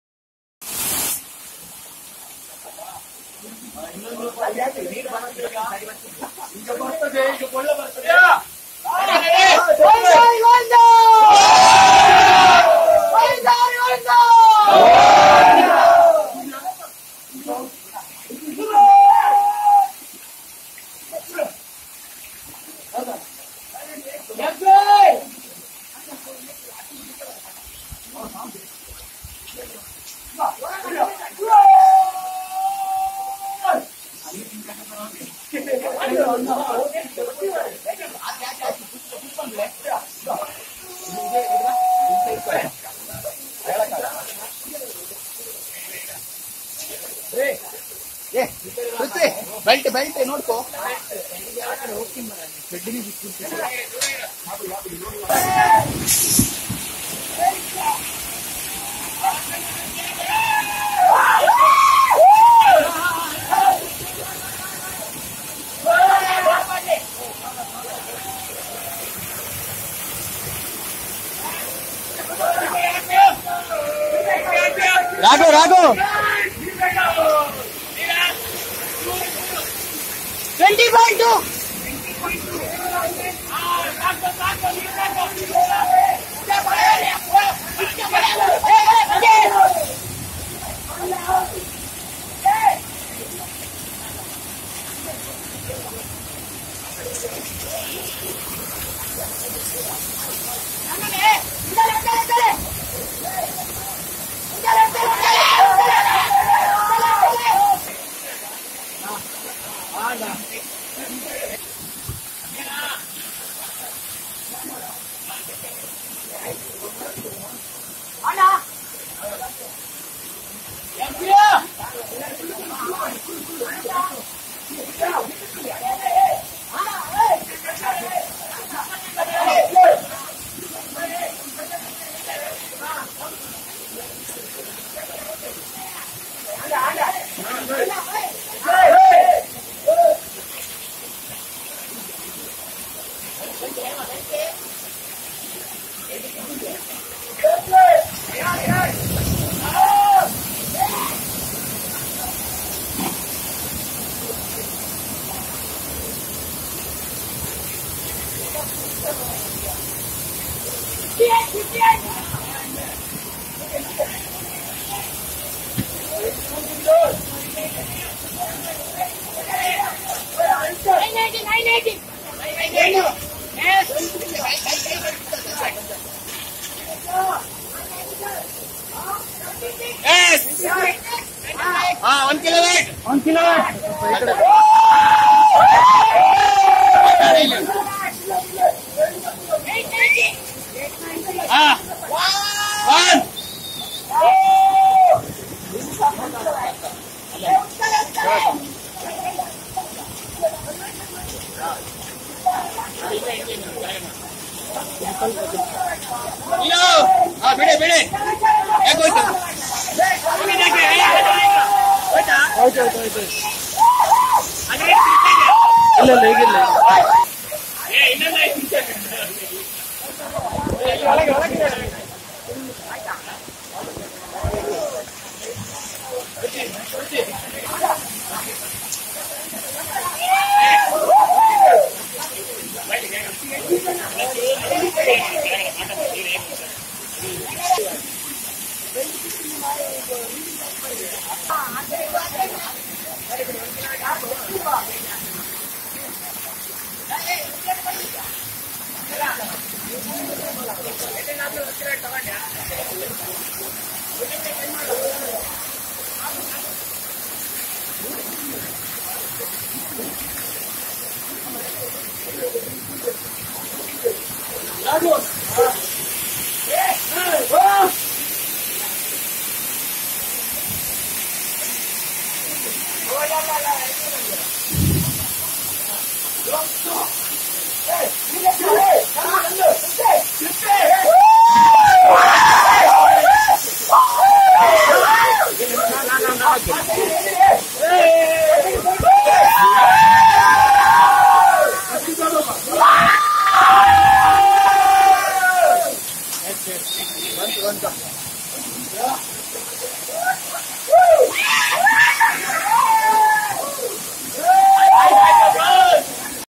Elliptical turbine- low head high discharge impulse turbine
Elliptical core having less moment of inertia, runs at higher RPM.works on both impulse and weight of water